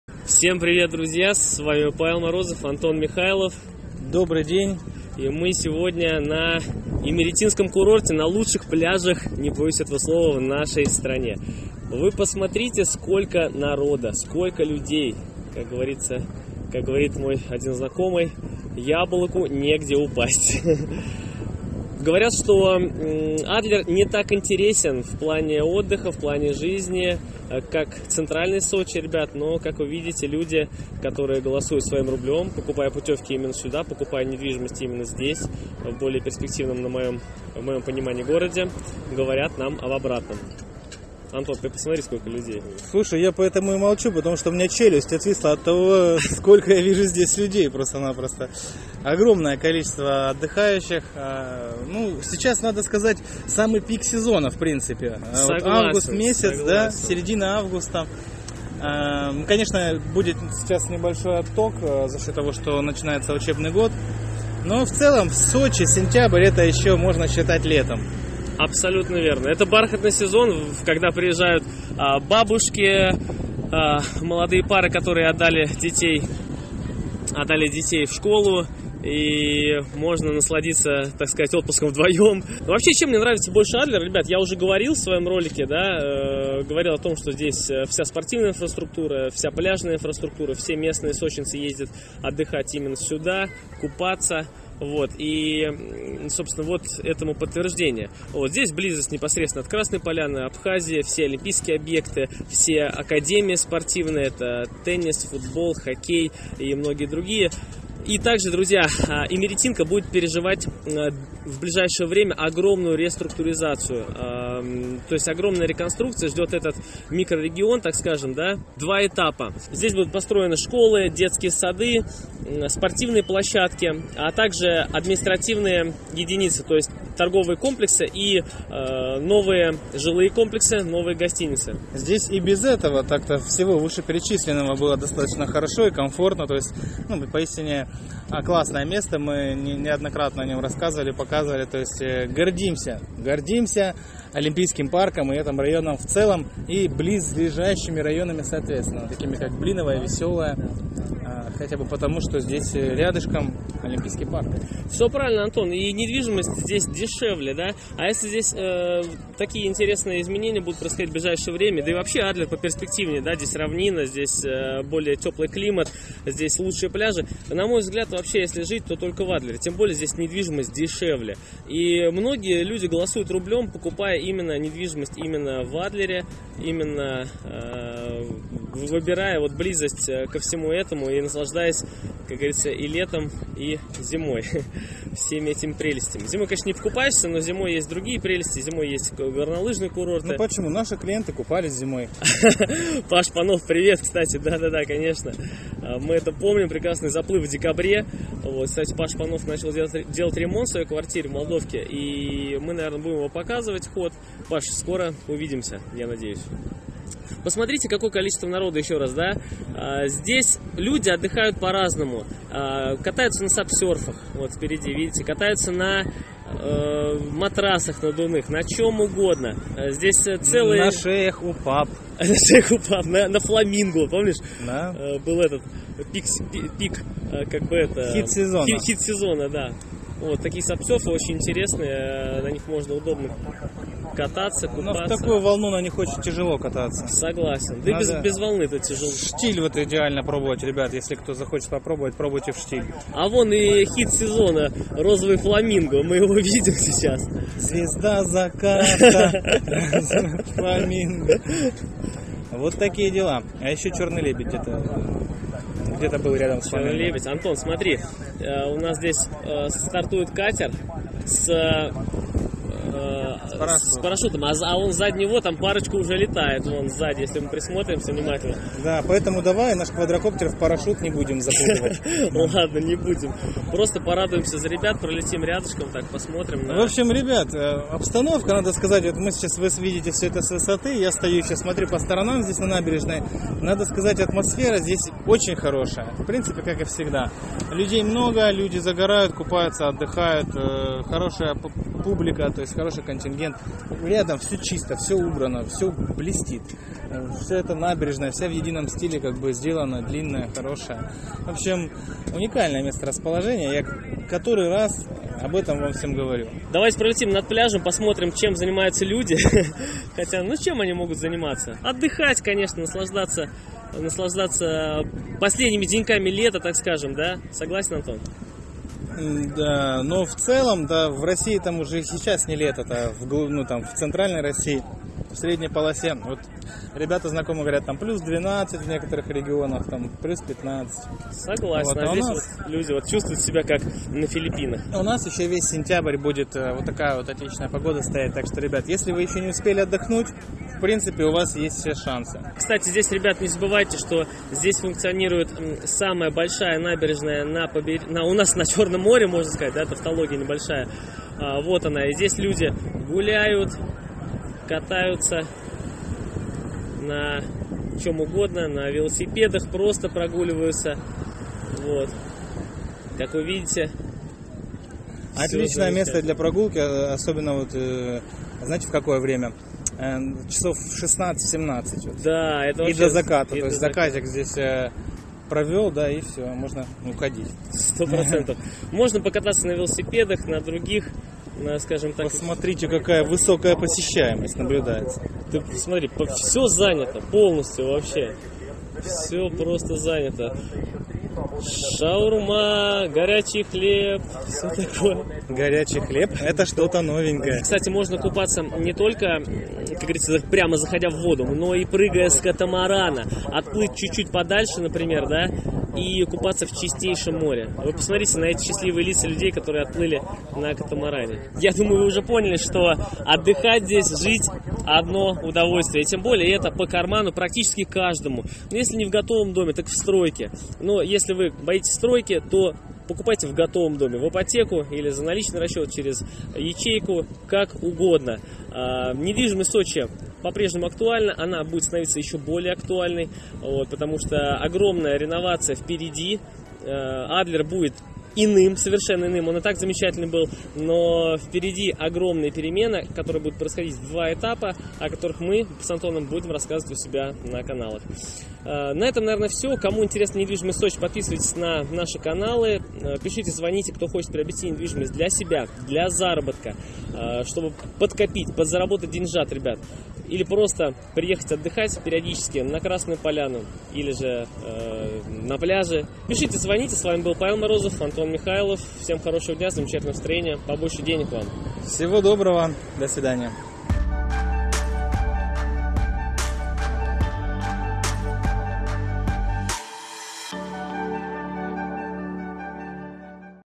🌊👙НА ПЛЯЖАХ МАЛО ТУРИСТОВ?.. 🌊👙 / ИМЕРЕТИНСКИЙ КУРОРТ СОЧИ 2019 / ПЛЯЖ ОЛИМПИЙСКОГО ПАРКА В СОЧИ
Друзья, сегодня в видео делаю обзор олимпийского пляжа с квадрокоптера.